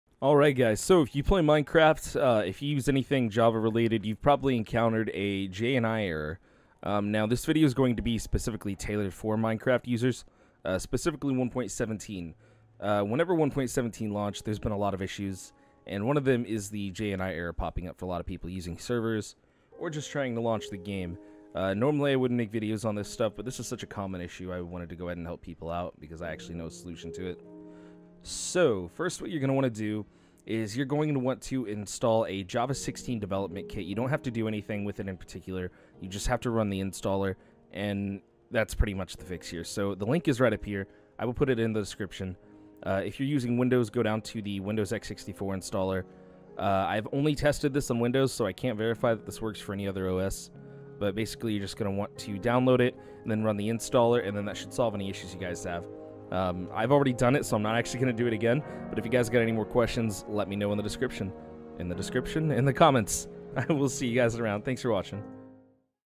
How to fix JNI Error on Minecraft 1.17 OR Java Applications
This is not my typical video, but I had this issue, myself, and found a solution pretty quickly. I made this video for those who may be struggling to find an answer simply by using a search engine, or for those who need a video as a guide. I hope you guys appreciate it! Thank you for stopping by!